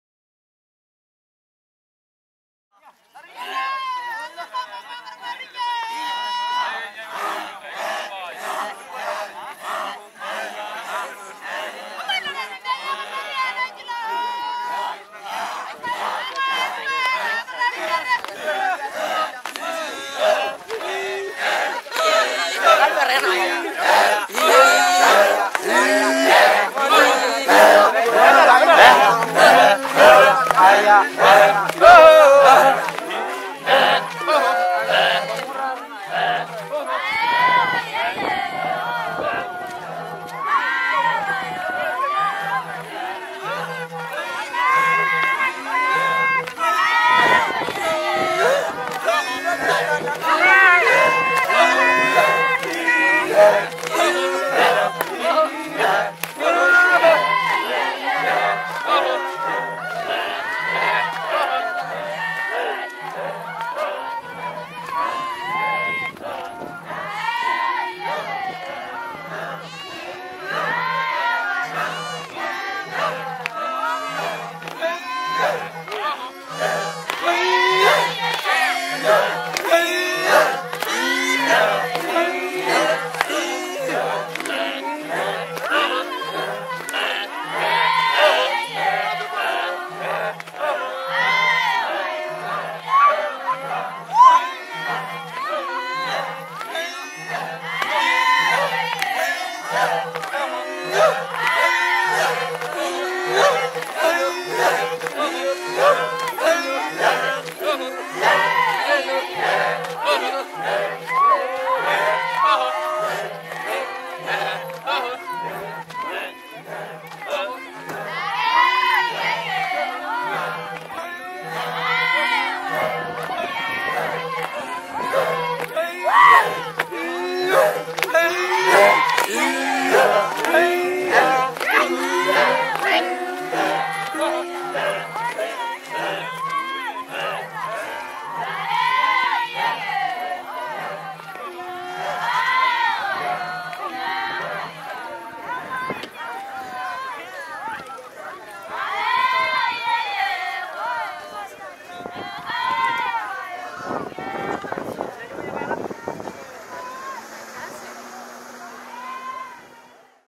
Maasai Dance 360 VR Experience Part 1
We Visited a Maasai camp in Tanzania and this is part of our experience in 360 degrees and Virtual Reality